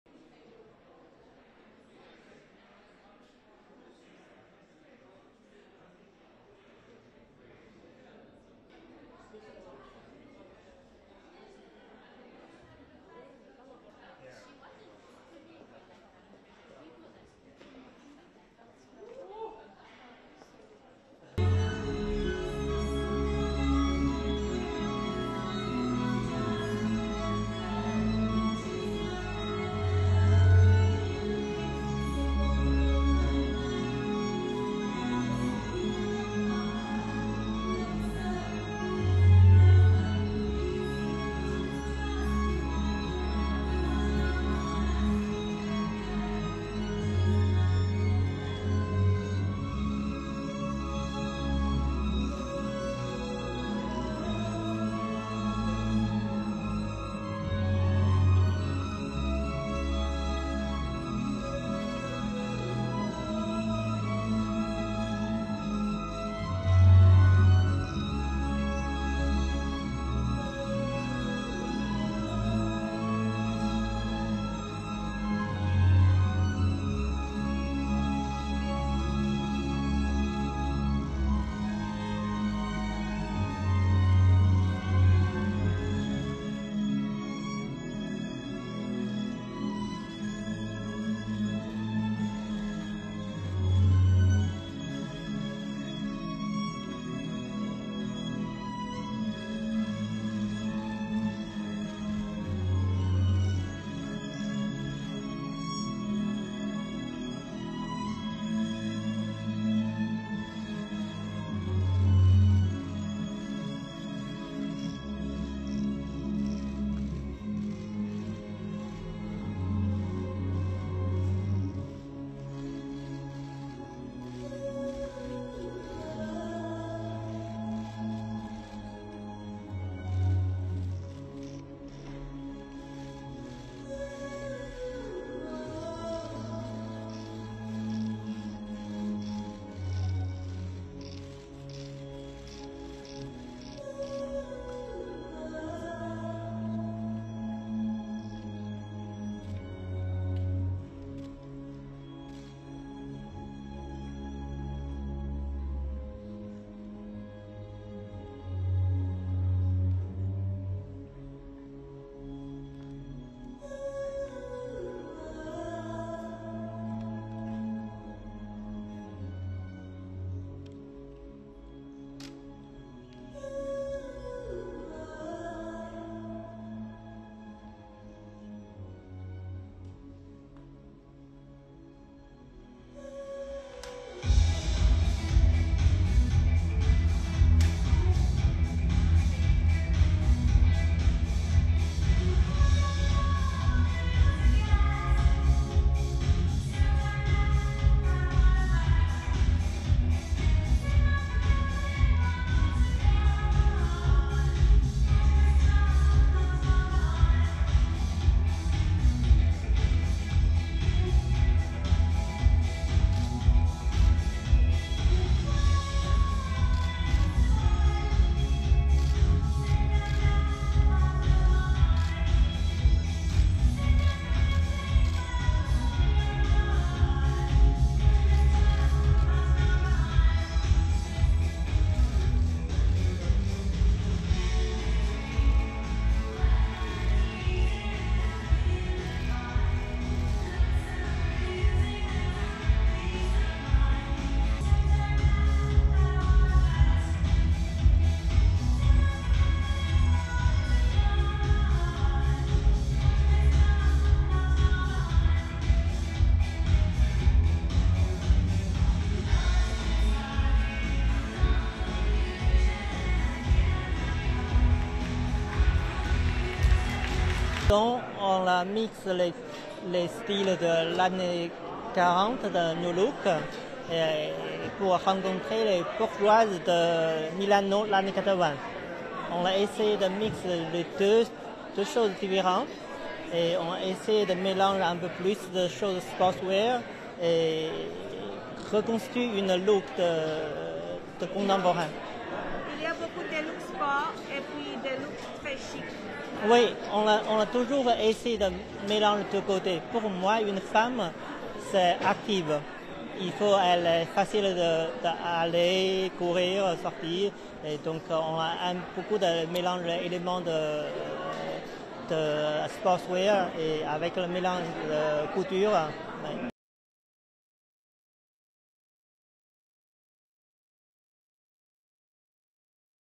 'Sportwear mixed with couture': Dawei shows latest collection in Paris
ASSOCIATED PRESS
Paris - 27 February 2024
1. Various of looks backstage
2. Wide shot models backstage
3. Various catwalk show
4. Cutaway crowd
4. Various final pas
6. SOUNDBITE (French) Dawei Sun, designer:
"We have mixed the style  of the '40s, the new look, which meets the (indecipherable) of Milan in the '80s. We tried to mix the two different things. And we've tried to mix sportswear items to recreate a contemporary look."
(Reporter: "There were a lot of sports looks as well very chic looks.")
"Yes, we have always tried to mix. For me a woman is active, it has to be easy for her to go run, to go out and so there are a lot of mixtures, elements of sportwear mixed with couture."
7. Medium shot  Dawei Sun
STORYLINE:
Dawei Sun showed his latest collection at Paris fashion week Tuesday.

Known for elegance and refinement interwoven with street style, this collection was no different.

Many of the looks featured specific tailoring, whether it be suits, jackets or trousers. One outfit paired a long pleated leather skirt with a leather overcoat.

The color palate was mainly neutrals, with a splash of burnt orange or forest green.